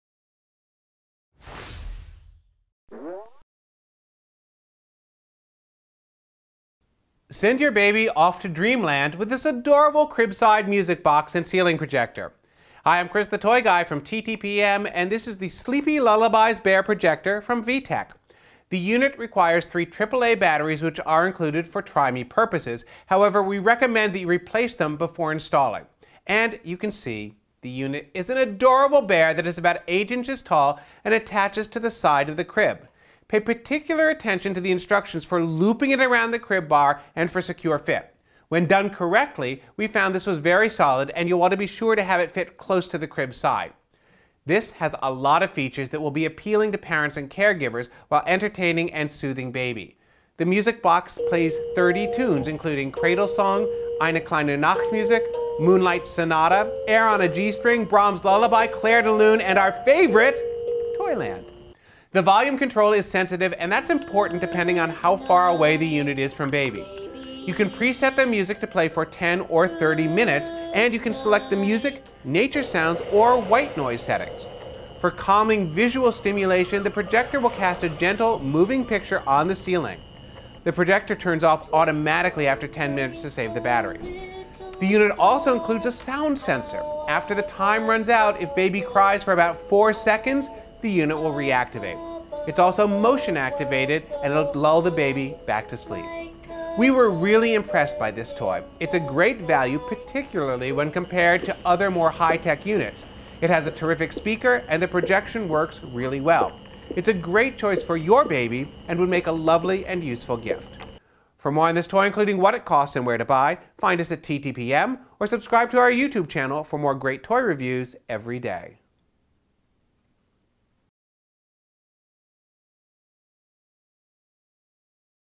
Sleepy Lullabies Bear Projector from VTech
Sleepy Lullabies Bear Projector attaches to cribs. Plays 30 different tunes and projects a soothing image on the ceiling. Set timers and volume. Sound and motion sensors re-activate unit if baby is restless. Well-made and a great value.

Product Info:
The Sleepy Lullabies Bear Projector from VTech is a crib side music box that plays 30 different bedtime-themed tunes. It also has a projector that will cast a soothing, colorful image on the ceiling. It attaches securely to the side of a crib, and it has a volume control, timer and sound and motion sensors to reactivate to lull a restless baby back to sleep.